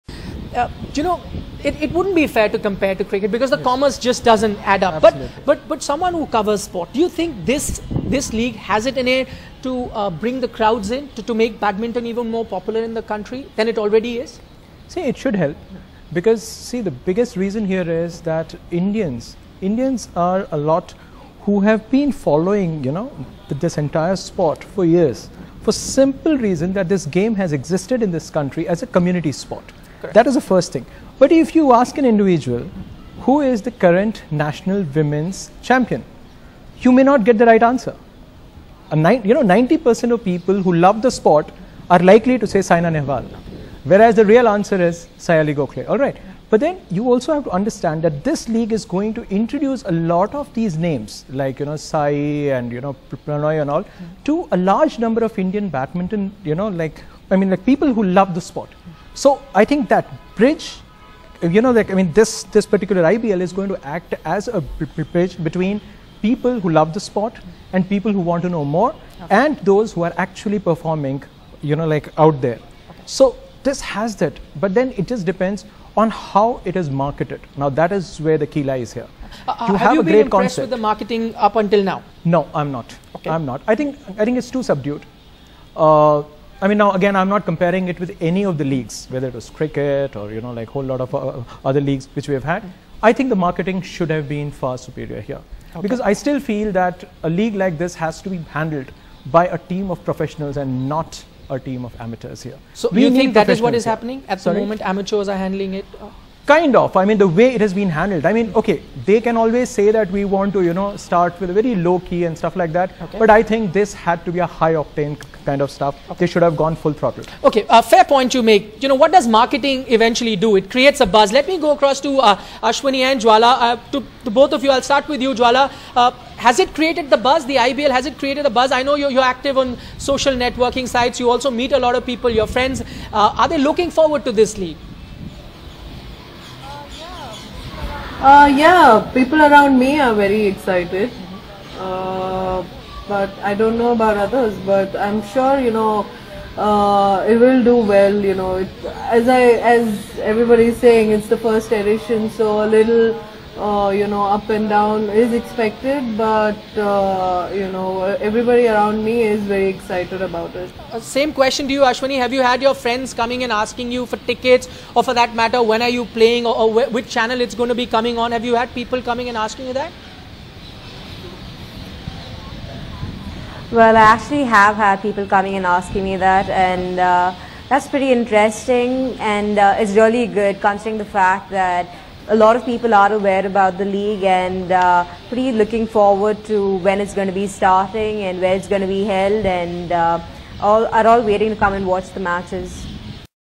Indian Badminton League set to fly. Can it match the glitz and glamour of an IPL?
The first edition of the Indian Badminton League starts in New Delhi on Wednesday. Six franchisees will vie for top honours in the million-dollar championship, spread over 18 days. With at least five top Indian players like World No.4 Saina Nehwal, current world bronze medallist PV Sindhu, former world doubles bronze medallist Jwala Gutta and Ashwini Ponnappa and London Olympics quarterfinalist Parupalli Kashyap in action, this will be a new beginning for Indian badminton. Can the championship match its cricketing counterpart? Opinions are divided, but it's all set for a gala start as all stake holders aim to ride a community sport that is yet to transform itself as an international event in the country. On day one of IBL, Delhi Smashers play Pune Pistons but the big fight will be on Independence Day when Sindhu (Awadhe Warriors) takes on Saina (Hyderabad Hotshots) in what will be a derby contest.